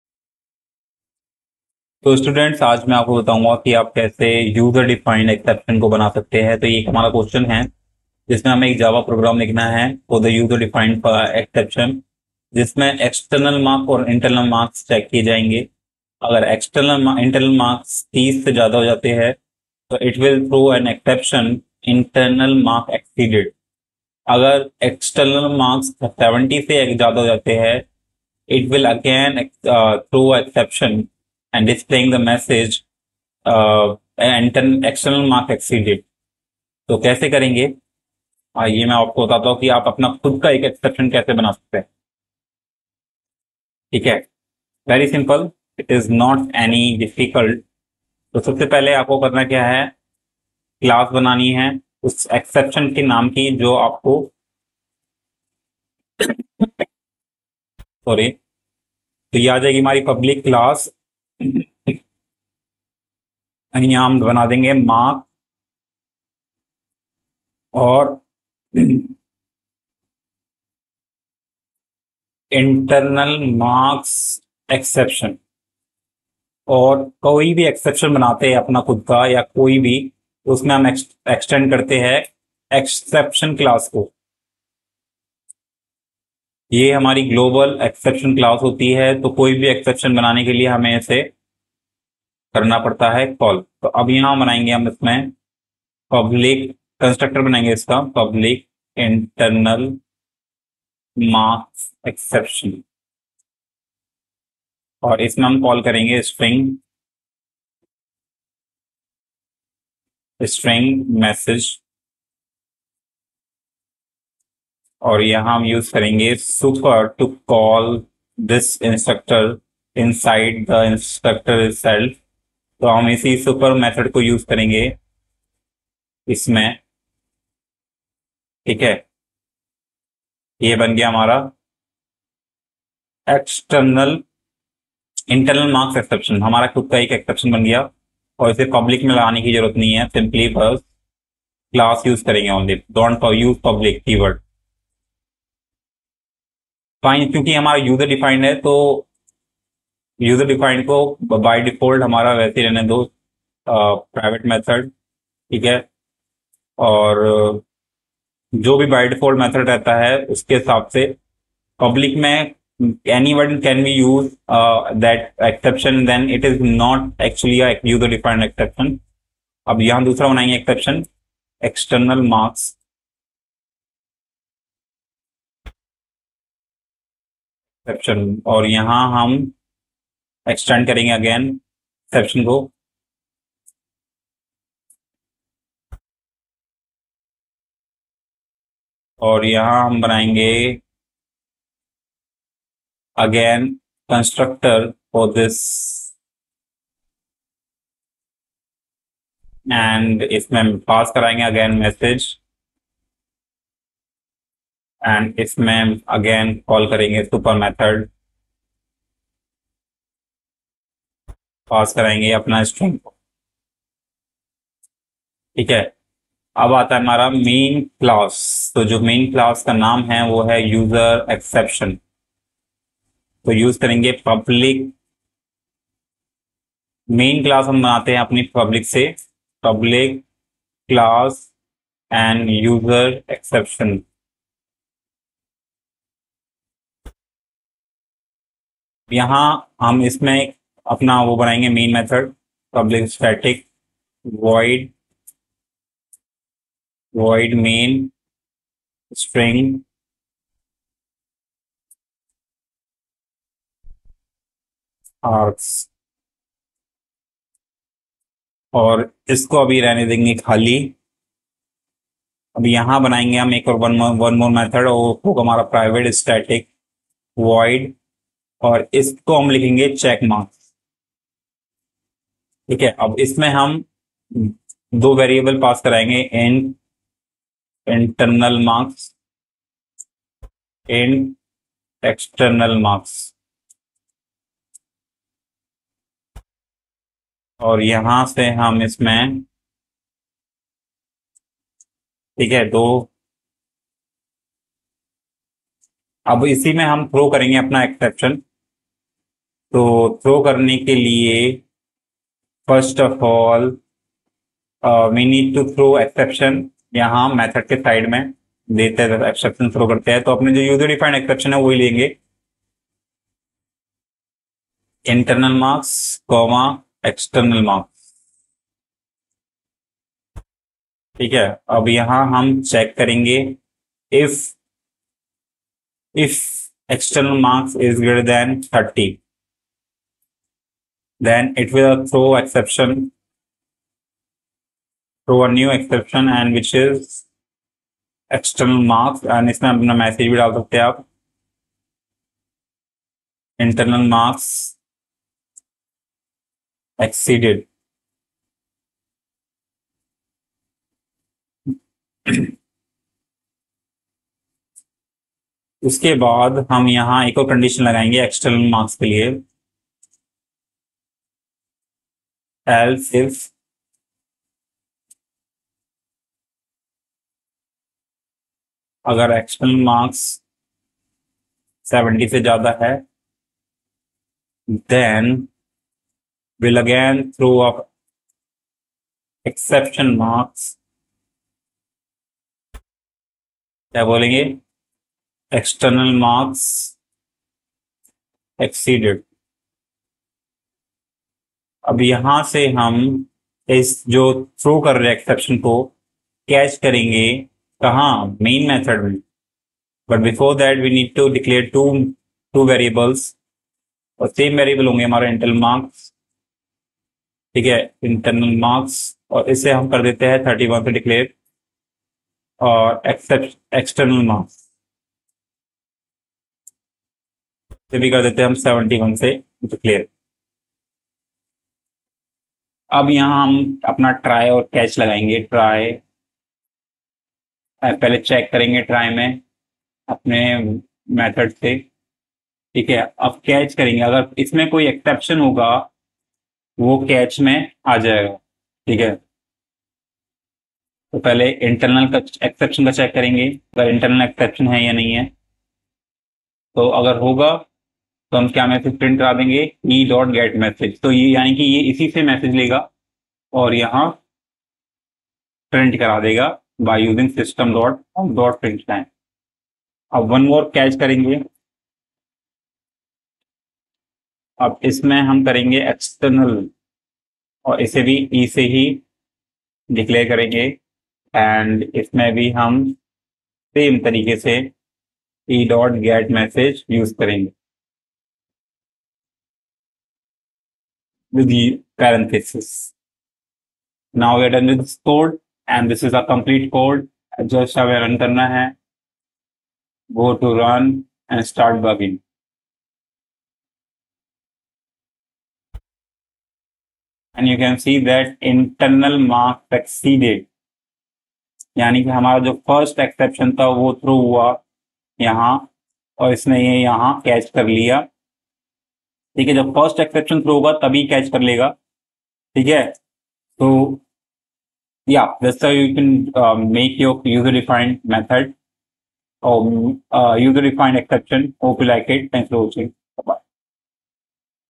Java Program - User defined exception | write a java program checks the internal and external marks
In this video, You will learn write a java program for user defined exception that checks the internal and external marks; if the internal marks is greater than 30 it raises the exception “internal mark exceeded”; if the external marks is greater than 70 it raises the exception and displays the message “external mark exceeded”, create the above exception and test the exceptions. please leave like if you want to see more video like